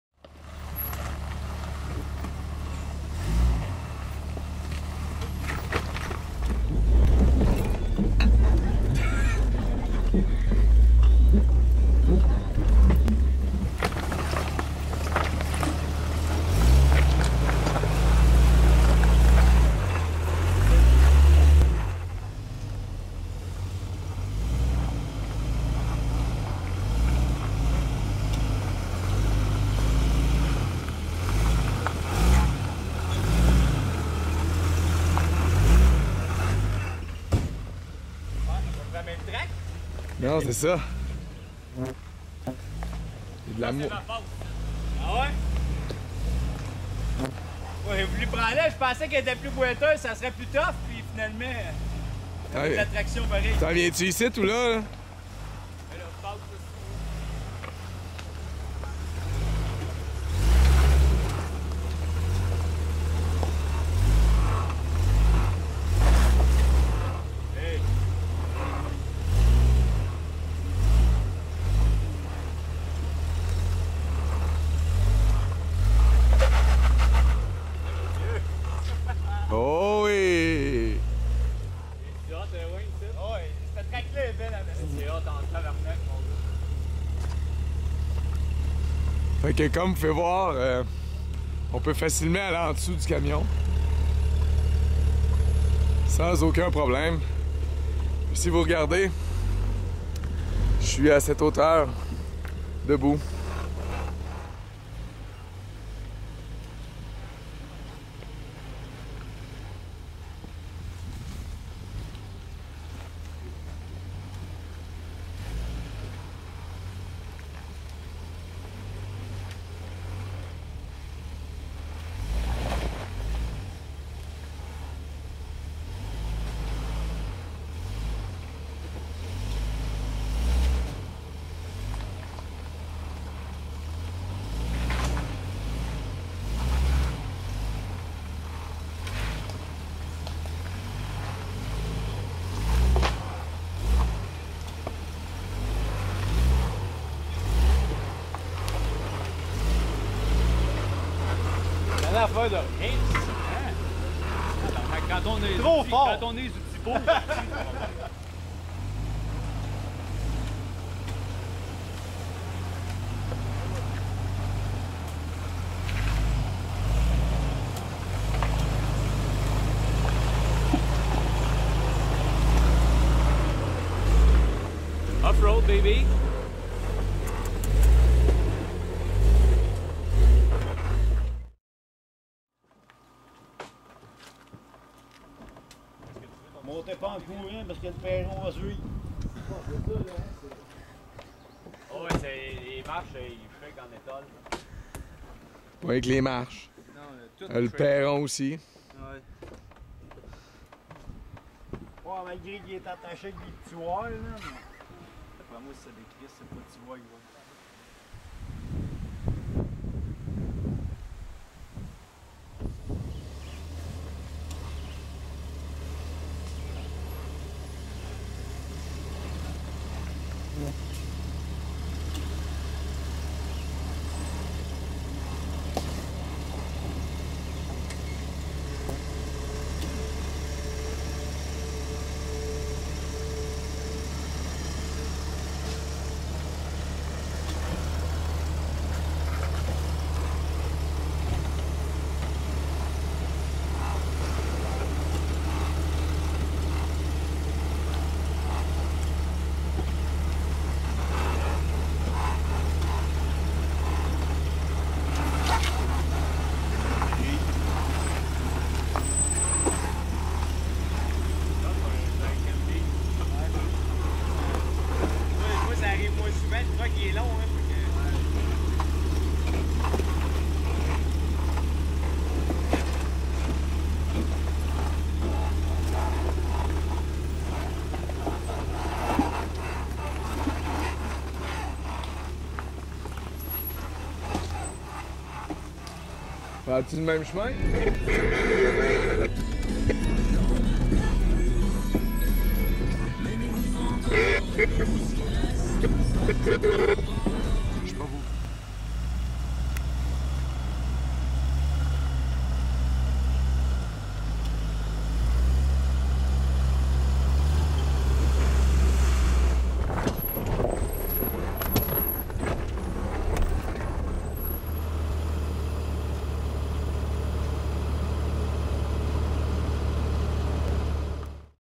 Jeep Cherokee, Belvedere 2010, offroad 4x4 trail
24 Juin 2010, randonné hors route dans le chemin du belvédère dans le gazoduc st-rock-de-Mekinac. Jeep Cherokee et GMC Sierra dans les sentiers de sable et roches qui mène à un magnifique panorama digne de la Mauricie. Quelques passages difficiles, mais ce ne serait pas une vraie journée de gars sans défis.

Vidéo: Roxan Turcotte
Caméras: Cédric Turcotte
Véhicule: Jeep Cherokee et GMC Sierra

In June 24th, 2010, gone an offroad ride in the way of the belvedere in the st-rock gas main of Mekinac. Cherokee jeep and GMC sierra in the paths of sand and rocks which leads to a magnificent panorama deserving of Mauricie. Some difficult passages, but it would not be the real guy's day without challenges.

Video: Roxan Turcotte
Cameras: Cédric Turcotte
Vehicle: Jeep Cherokee et GMC Sierra

You can also Watch:
Vous pouvez aussi regarder: